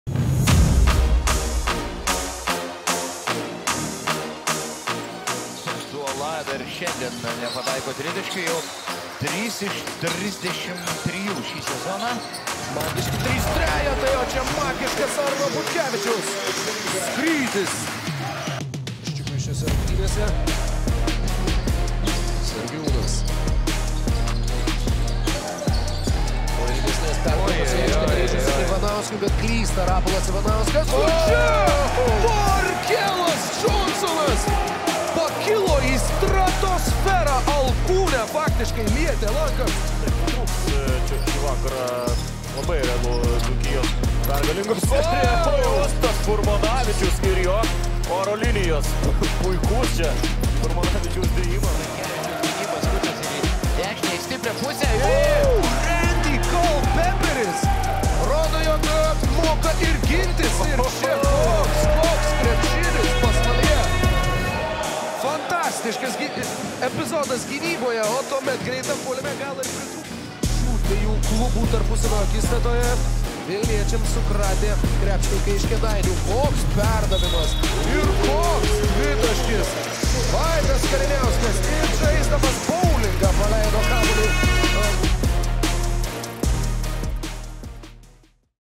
Gražiausias savaitės momentas – nuostabus V. Kariniausko perdavimas per visą aikštę
„Betsafe-LKL“ čempionate prabėgus dar vienai savaitei, portalas LKL.lt aistruoliams pristato gražiausių savaitės momentų rinkinį.

Į įspūdingiausių epizodų penketuką šįkart pateko Arno Butkevičiaus ir Justo Furmanavičiaus dėjimai, du Pasvalio „Pieno žvaigždžių“ legionierių sukurti momentai, o pirmosios vietos laurus šįkart susižėrė Vaidas Kariniauskas, atlikęs meistrišką perdavimą per visą aikštę Lukui Uleckui, kuris komandos draugo perduotą kamuolį pasiuntę į krepšį su varžovo pražanga.